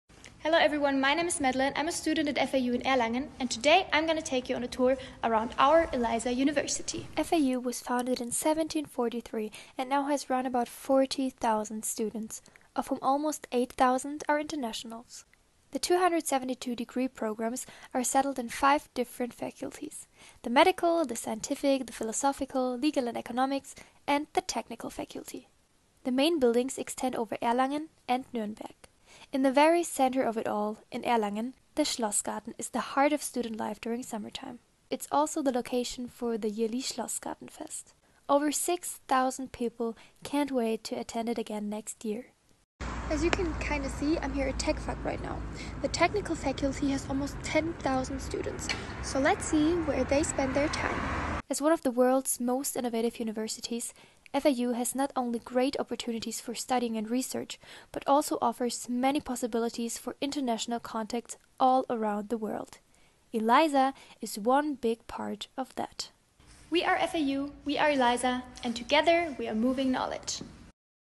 We are FAU, We are EELISA, together we are moving knowledge [FAU Programme]
Reise mit EELISA durch ganz Europa!

Wenn du ein EELISAStudi bist, laden wir dich zu deinem nächsten Abenteuer im Ausland ein: erweitere dein Netzwerk in einer mehrsprachigen und vielfältigen Umgebung! Wähle zwischen Madrid, Pisa, Paris, Istanbul, Budapest, Bukarest, Zürich - oder Erlangen-Nürnberg!

Wie wäre es mit einem Semester an der @uni_fau in Deutschland? Madelaine zeigt dir die Uni!

Sieh dir den EELISA-Kurskatalog an und leg los!
(english version and links below)

Travel all around Europe with EELISA!
If you are an EELISA-Student we invite you to your next adventure abroad to expand your network in multilingual and diverse environments. Choose between Madrid, Pisa, Paris, Istanbul, Budapest, Bucharest, Zurich - and Erlangen-Nuremberg!

How about a semester at FAU in Germany? Madelaine shows you around.

To get started, check the EELISA Course Catalogue!